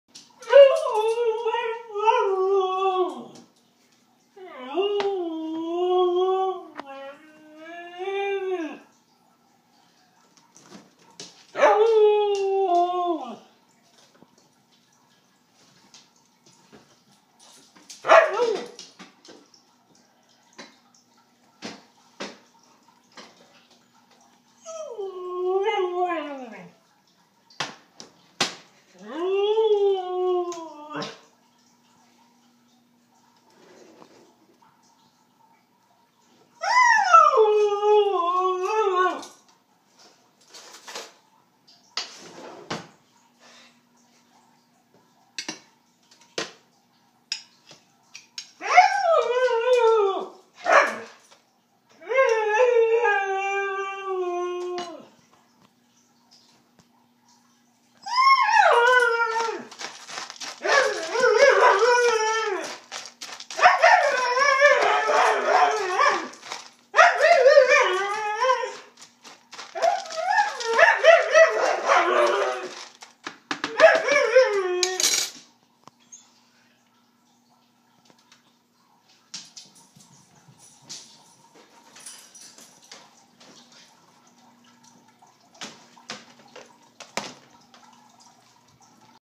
Ruby Siberian Husky Dog Talking
Ruby being vocal about my preparation of her dinner.